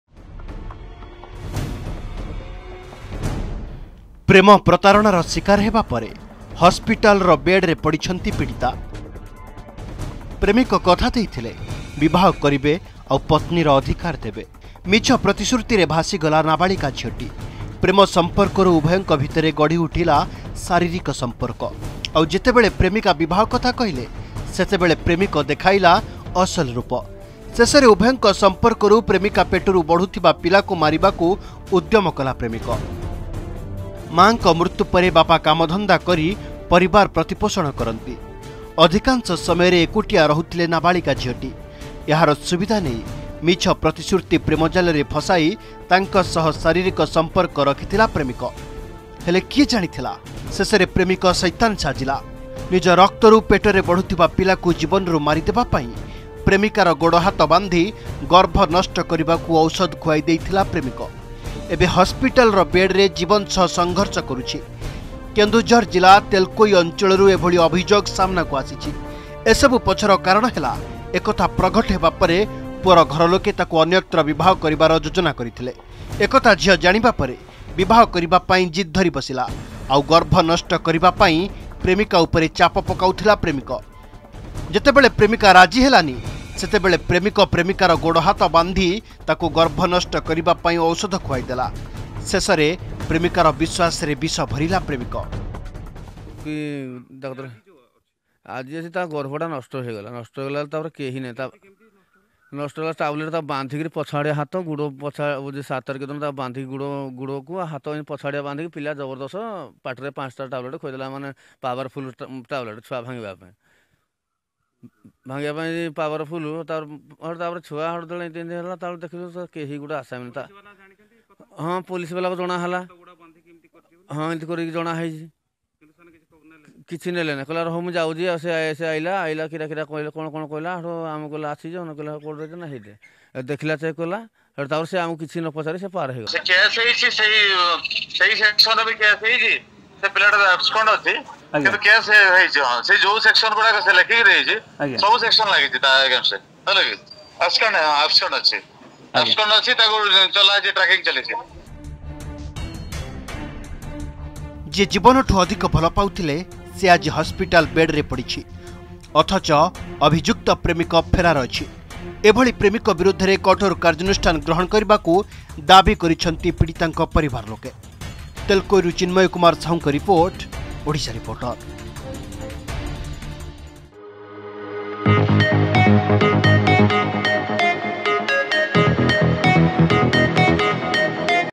ଏଭଳି ପ୍ରେମିକକୁ କି ଶାସ୍ତି ଦରକାର ? | Odisha Reporter
ପ୍ରେମ ପ୍ରତାରଣାର ଶିକାର ହେବା ପରେ ହସ୍ପିଟାଲରେ ବେଡରେ ପଡ଼ିଛନ୍ତି ପୀଡ଼ିତା । ପ୍ରେମିକ କଥା ଦେଇଥିଲେ ବିବାହ କରିବେ ଆଉ ପତ୍ନୀର ଅଧିକାର ଦେବେ। ମିଛ ପ୍ରତିଶୃତିରେ ଭାସିଗଲା ନାବାଳିକା ଝିଅଟି। ପ୍ରେମ ସଂପର୍କରୁ ଉଭୟଙ୍କ  ଭିତରେ ଗଢ଼ିଉଠିଲା ଶାରୀରିକ ସଂପର୍କ। ଆଉ ଯେତେବେଳେ ପ୍ରେମିକା ବିବାହ କଥା କହିଲେ, ସେତେବେଳେ ପ୍ରେମିକ ଦେଖାଇଲା ଅସଲ ରୂପ।